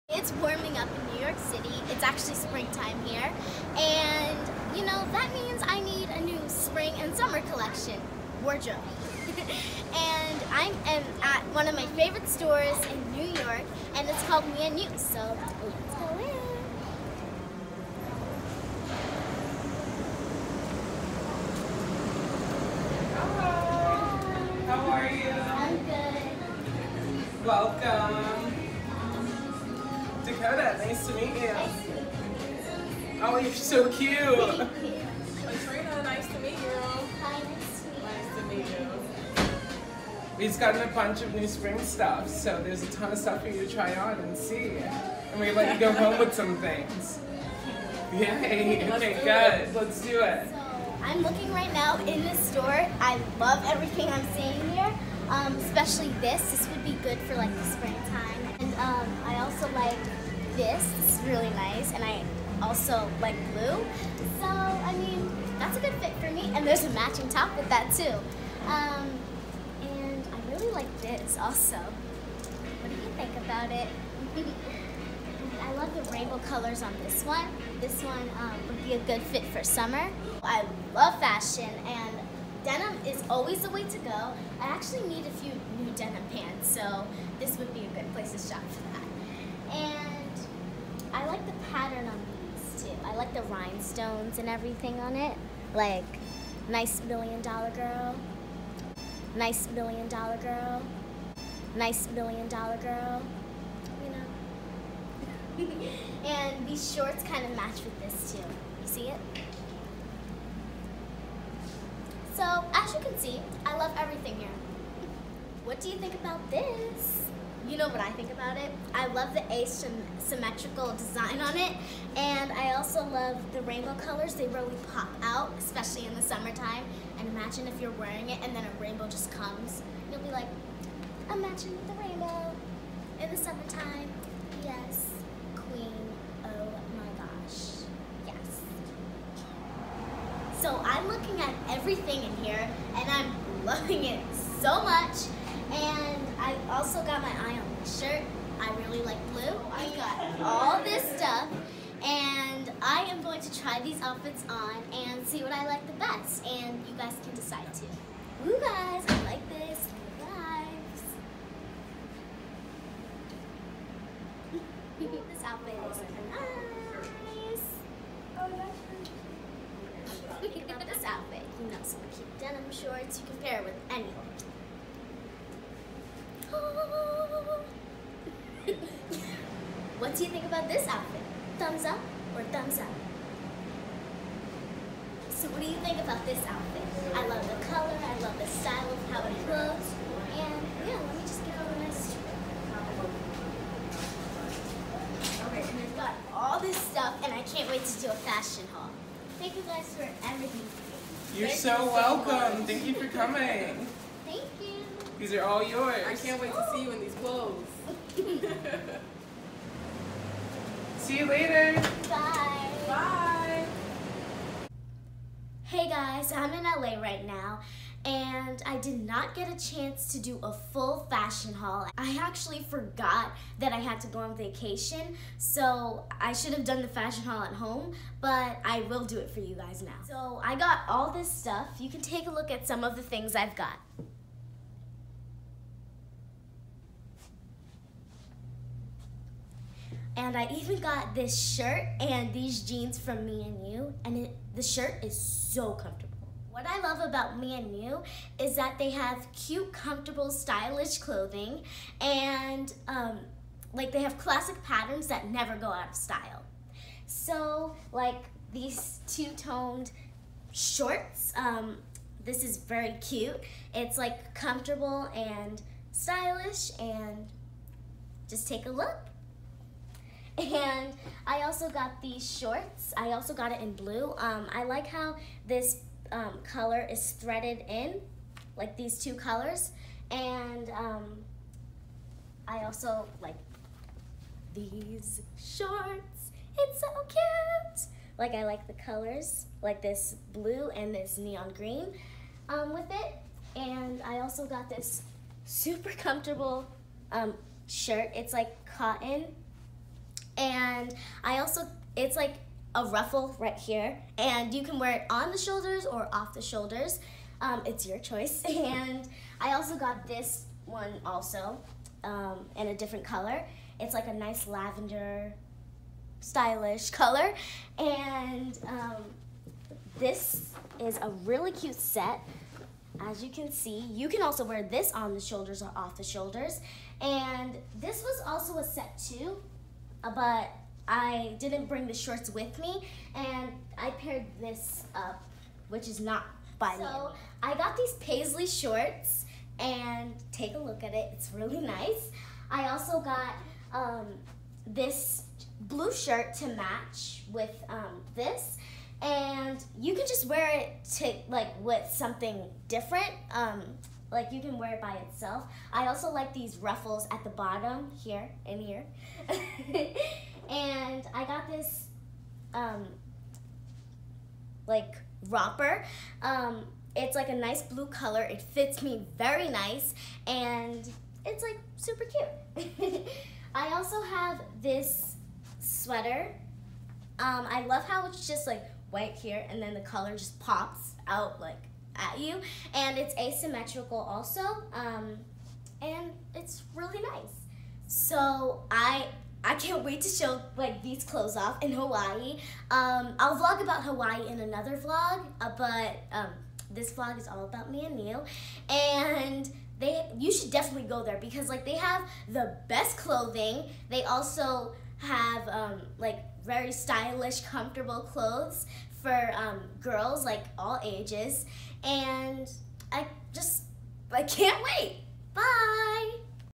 Vlog: Let’s Go Shopping At One Of My Favorite Stores - Me. N. U
Come along with me as I take a trip to one of my all time favorite stores, in NYC, Me.N.U  and look for a new Spring and Summer wardrobe. Me.N.U (pronounced Me and You) is located in NYC at 1042 Lexington Ave, New York, NY. They are also online at me-n-u.com